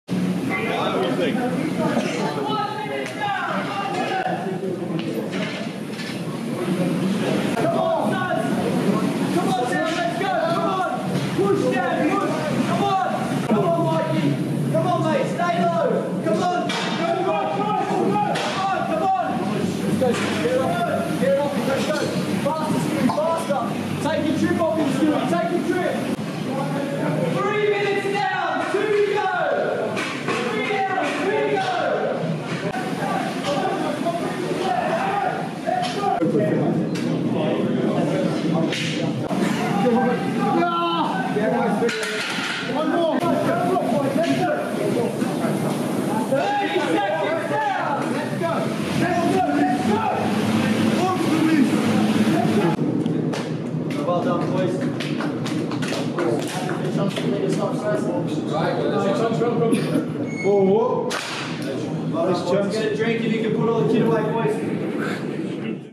Inside Training | Strongman session
Listen in as the squad wrap up pre-season week 4 with an intense strongman session!